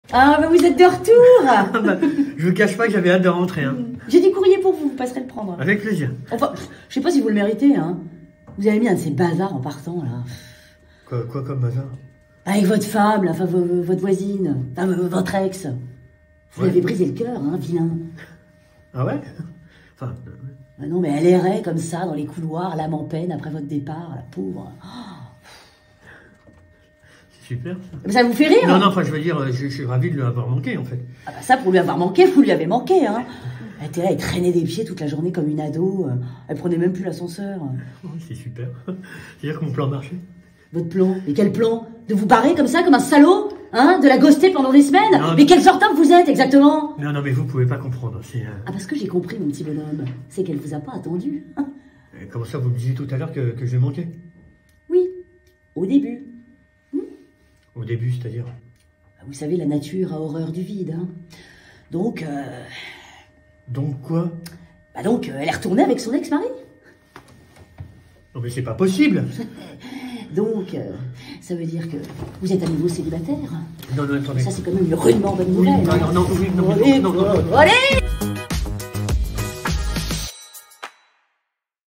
S3/EP1: Le Retour avec Énora Malagré
Après 6 semaines d’absence, le voisin revient enfin dans l’ascenseur.
Mais la gardienne @enoramalagre va lui apprendre une terrible nouvelle.